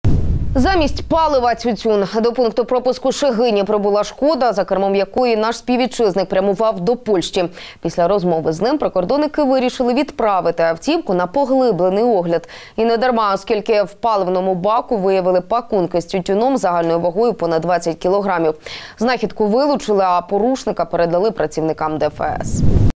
Замість палива тютюн. ПравдаТУТ Львів
Замість палива тютюн. До пункту пропуску "Шегині" прибула "Шкода", за кермом якої наш співвітчизник прямував до Польші. Після розмови з ним прикордонники вирішили відправити автівку на поглибленний огляд. І не дарма, оскільки, у паливному баку виявили пакунки з тютюном загальною вагою понад 20 кілограмів. Знахідку вилучили, а порушника передали працівникам ДФС.